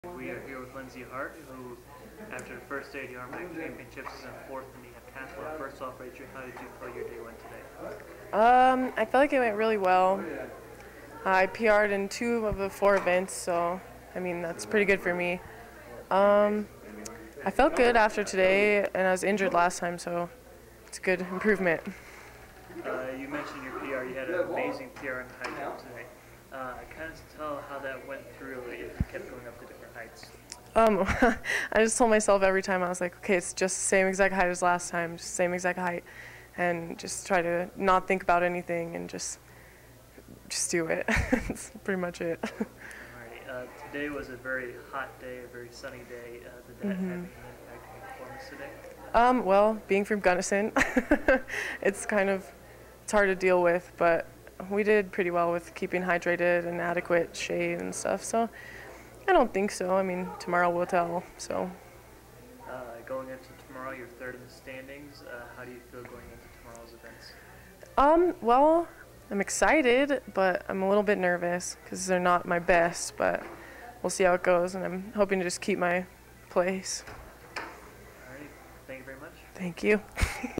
RMAC Outdoor Day 1 - Lindsay Hart
Western State College student-athlete Lindsay Hart talks about the first day of the heptathlon at the 2011 RMAC Outdoor Track and Field Championships.